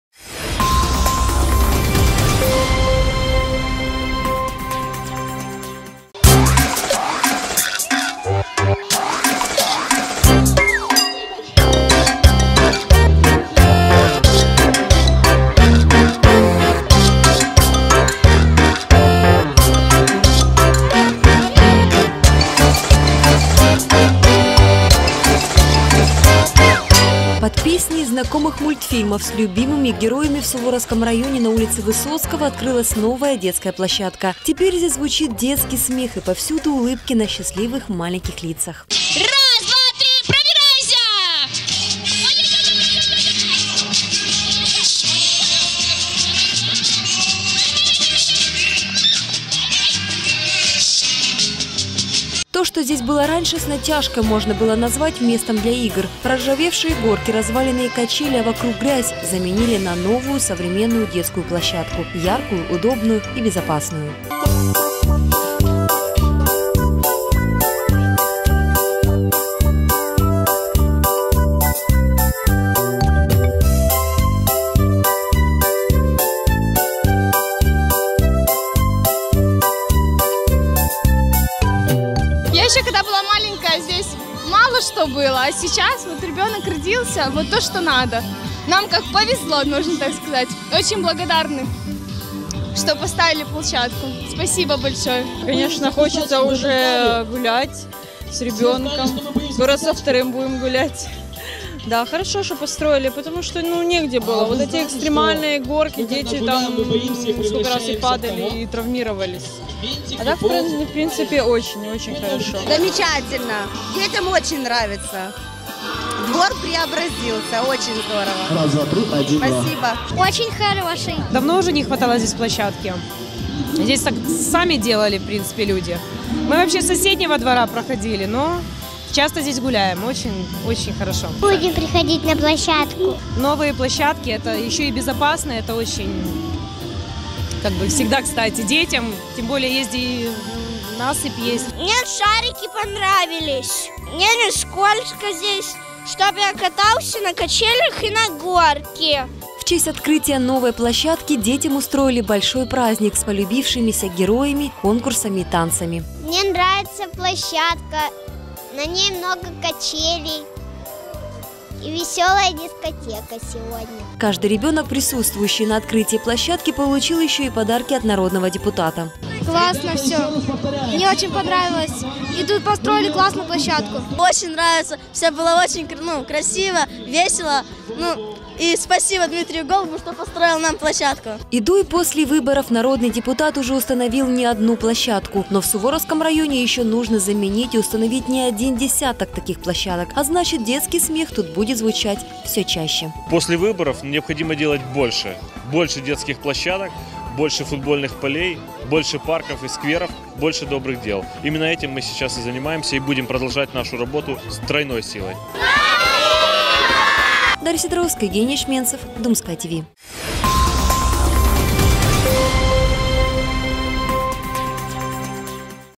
Лучшее – детям
Под песни из знакомых мультфильмов с любимыми героями в Суворовском районе на улице Высоцкого открылась новая детская площадка. Теперь здесь звучит детский смех и повсюду  улыбки на счастливых маленьких лицах.